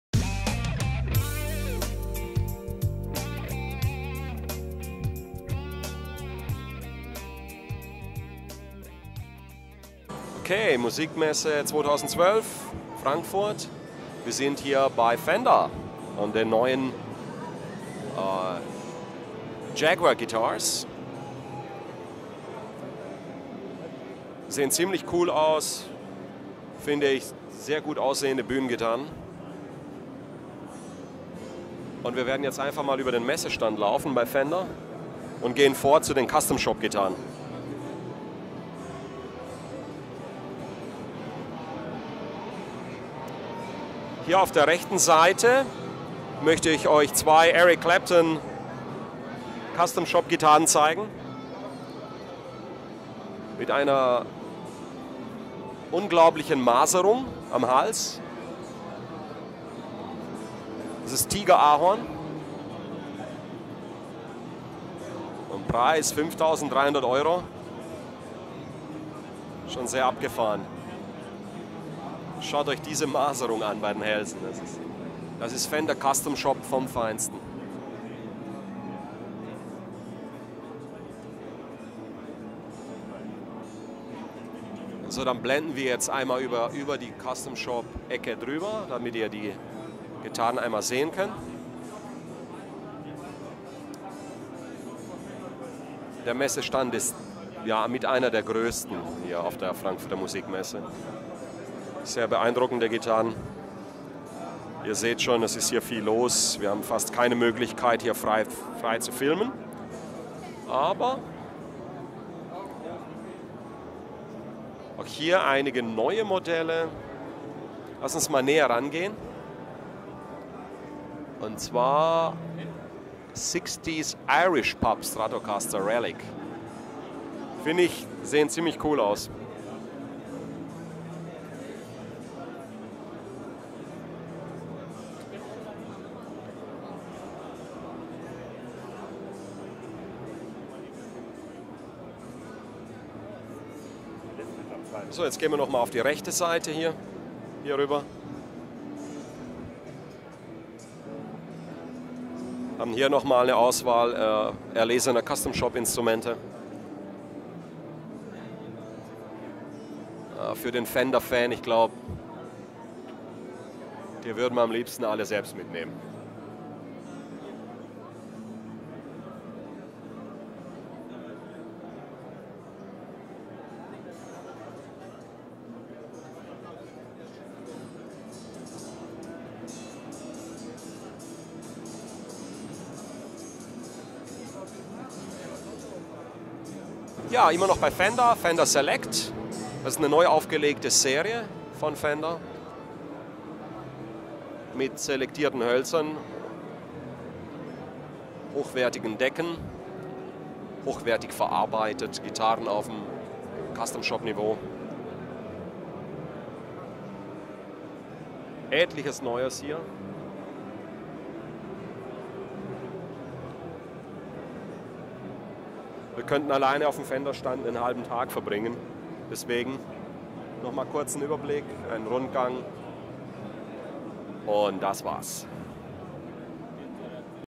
Musikmesse 2012: "Fender Musical Instruments"
VIP-Guitar auf der Musikmesse 2012. Eindrücke, Tests, Kommentare und Berichte von der Musikmesse Pro Light & Sound in Frankfurt a.M.